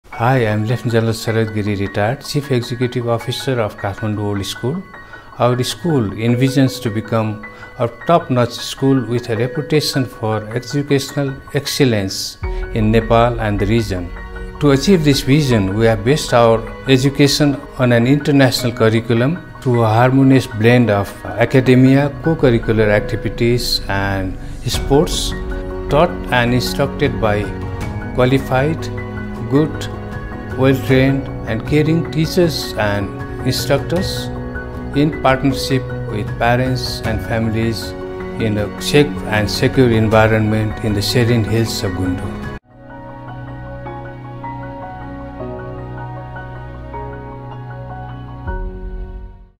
Lieutenant General Sarad Giri (Retired), Chief Executive Officer of Kathmandu World School
"To achieve this vision we have based our education on an international curriculum through a harmonious blend of academia co-curricular activities and sports taught and instructed by qualified, good, well trained, and caring teachers and instructors in partnership with parents and families in a safe and secure environment in the serene hills of Gundu."
-  Lieutenant General Sarad Giri (Retired),
   Chief Executive Officer of Kathmandu World School